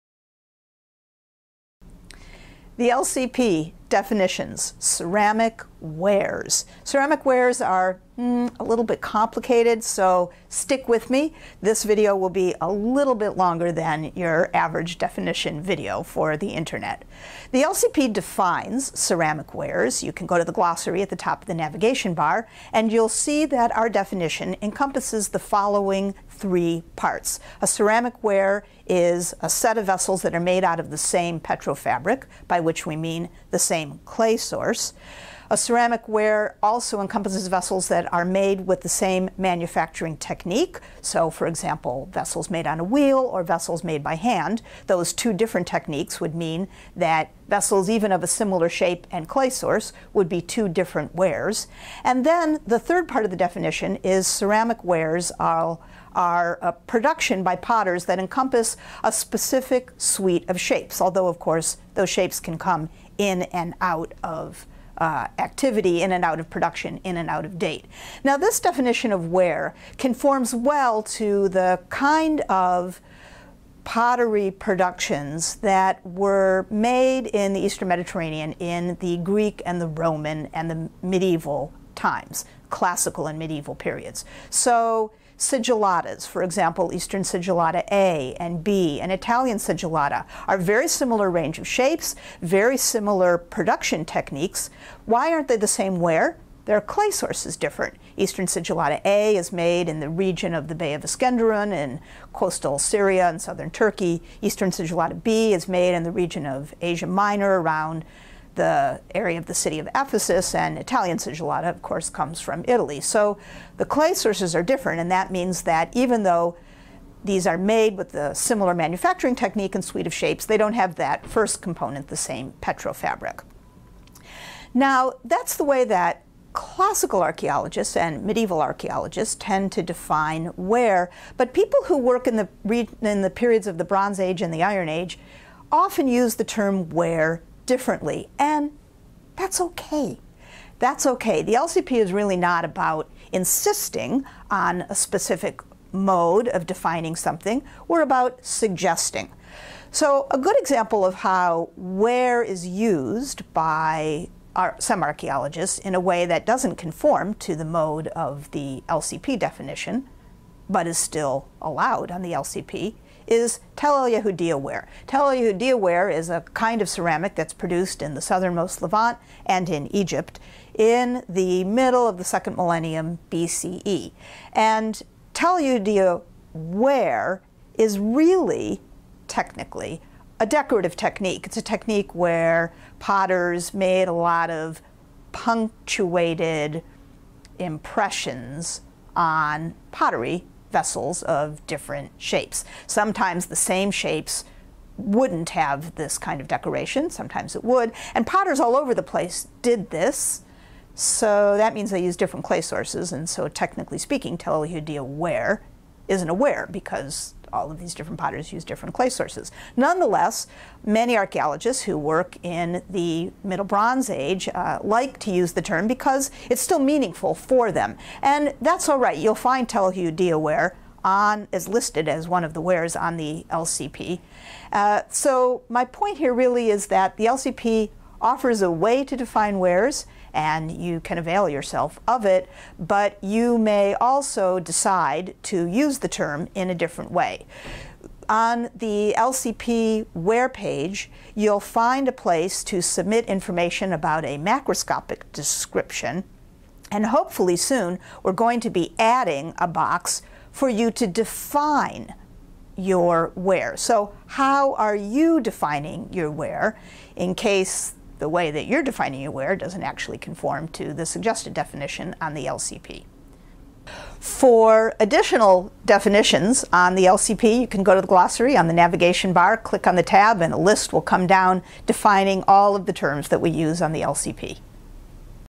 What is a Ceramic Ware?
In this video we explain how the Levantine Ceramic Project defines a ceramic ware.